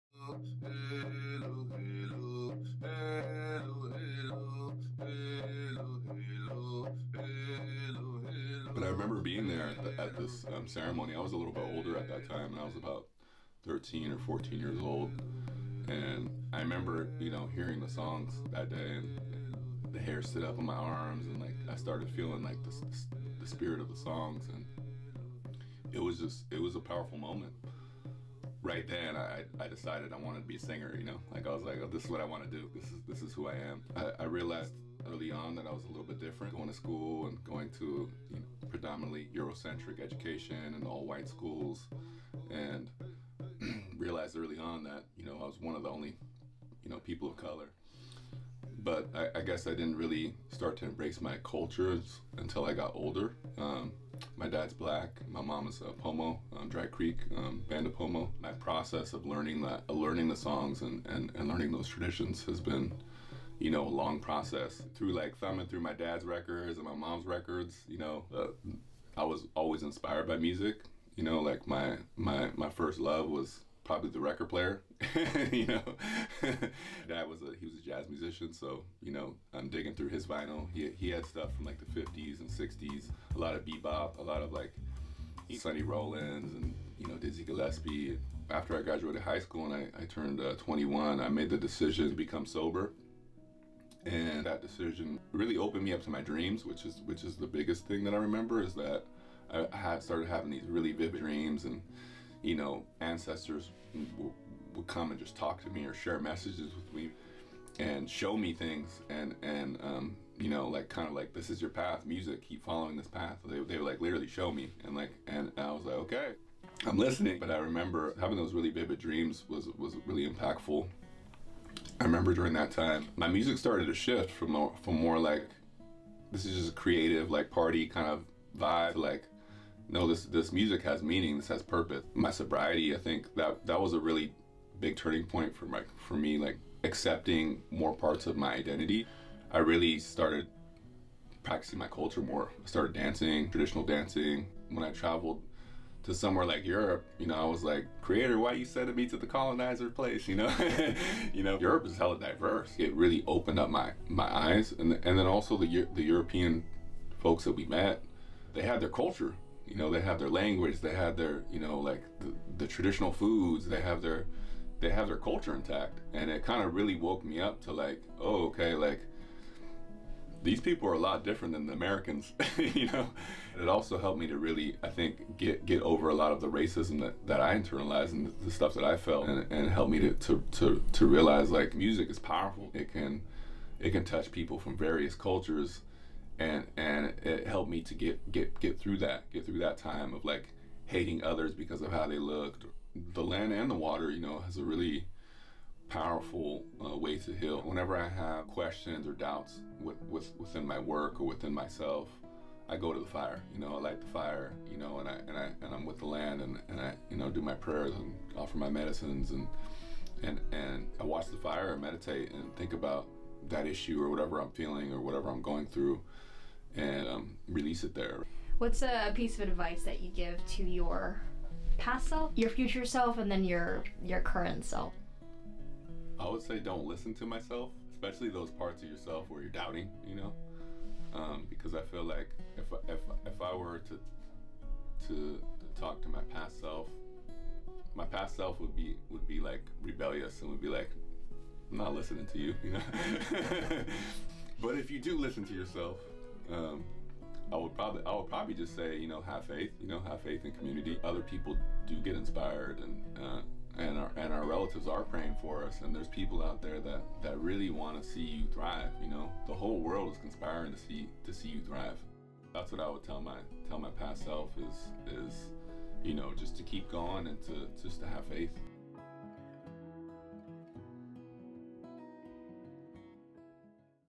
Ras K'Dee: Finding His Music
A short documentary focused on Bay Area musician Ras K'Dee who talks about how his cultural roots influenced his music.